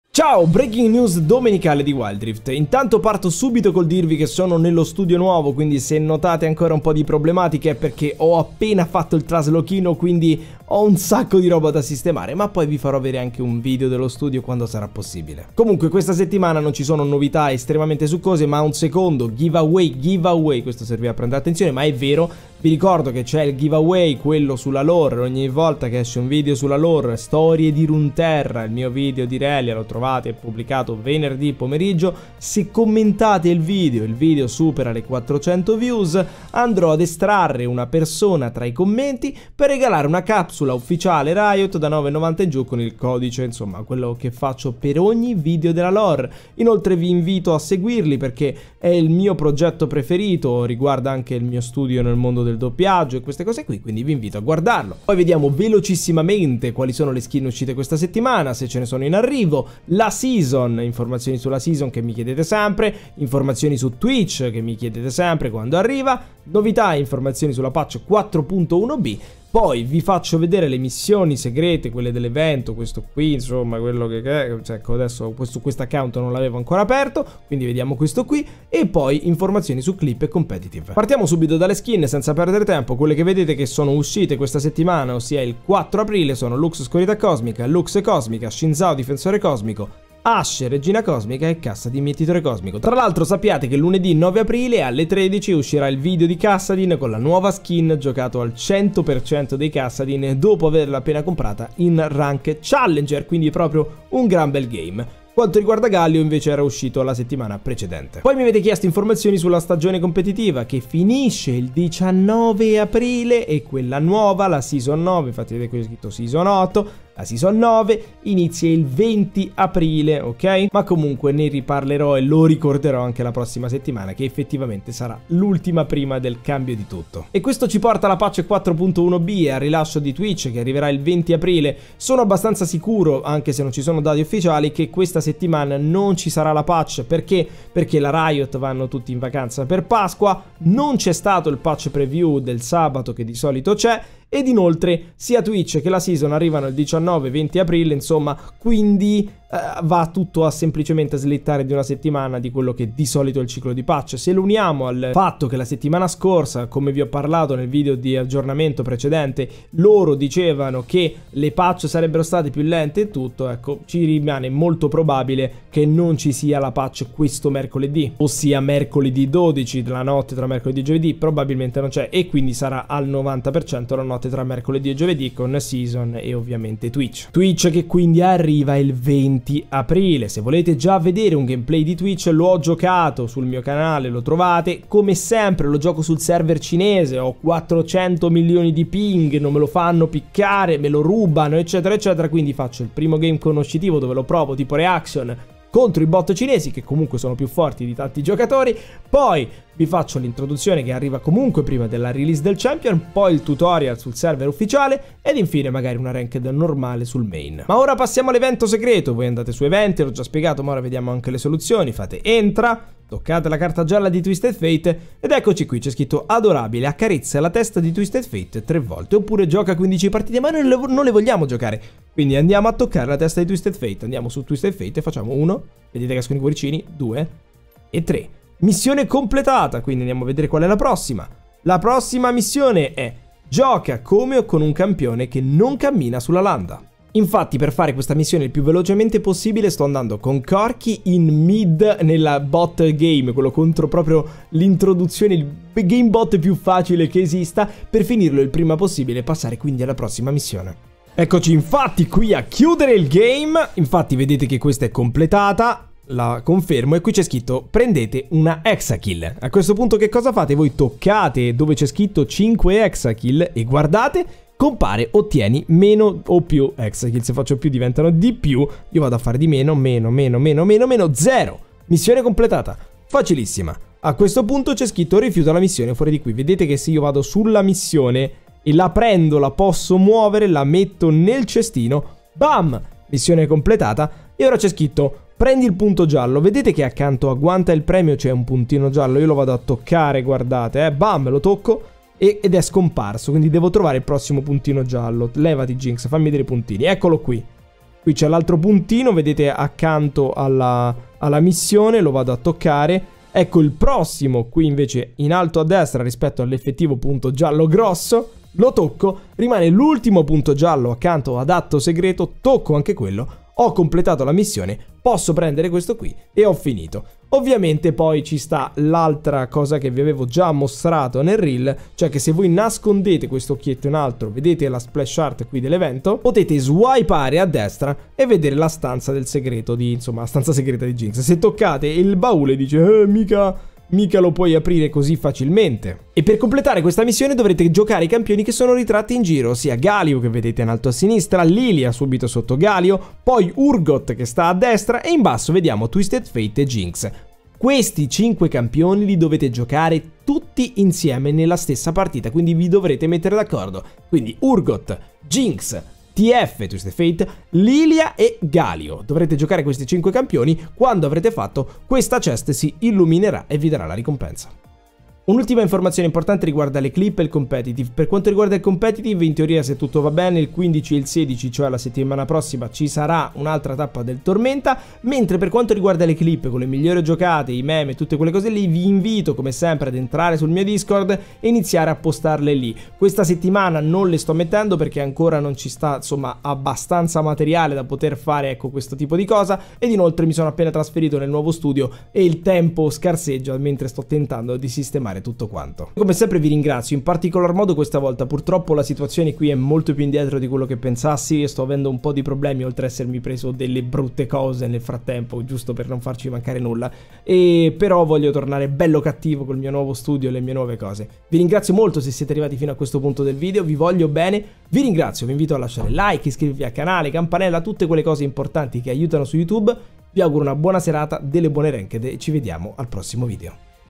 Wild Rift News: Rilascio Twitch, Nuova Season e Soluzioni Evento Segreto! Un breve Recap!
Con Jumpy Allen Wild Rift Ita

Mi raccomando con le giocate sul Discord!
Buona Pasqua!

𝐂𝐎𝐋𝐋𝐀𝐁𝐎𝐑𝐀𝐙𝐈𝐎𝐍𝐈